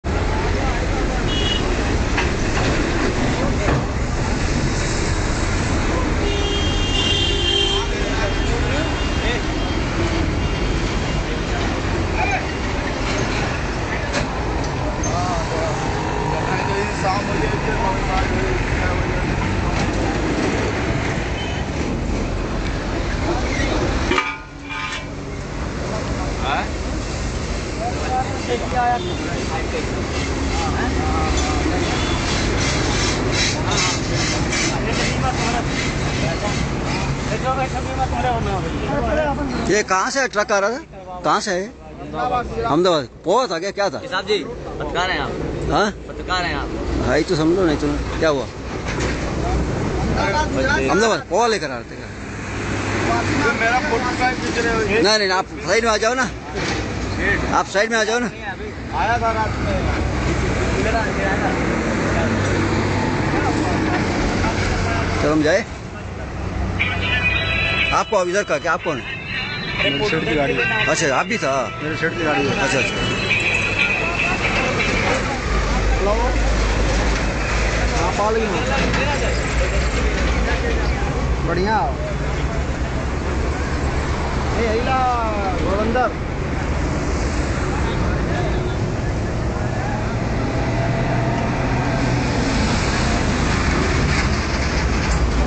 IT IS INJURIOUS TO SEE WATCH THIS VIDEO.MP4
Look at the hectic crisis management at the truck accident site today. Thanks to mobile almost every body is calling up their contacts...
NOW: truck plunged  over highway bridge construction site yesterday (Sept 14 night)
THEN on 14th June truck plunged into river, off the same bridge
Here's exclusive truck accident photo, once again  at Kaman Bridge on NH8. The truck  driver refused to talk and tell his  name but felt relieved as he is safe. The truck was carrying food stuffs from Gujarat and the truck skit at 1.30 am midnight.. There was no rain but he blames a sudden jerk caused due to potholes on the bridge. The truck owners supervisor had reached the site today morning at 7.30 am and thanked god as all 3 were unhurt.. The constuction  site supervisor also thanked god because it happened in the night. Had it happened during the day it would have caused some casualties as you see in the pic, the strong iron poles and a strong diesel  engine prevented the truck from further disaster.
This is the same place where on 14 th June early morning another truck had plunged  into a small river or a big nullah from this notorious bridge on NH8. POSTING BY SHRIPAD BHAT see my eye catching vides on yutube.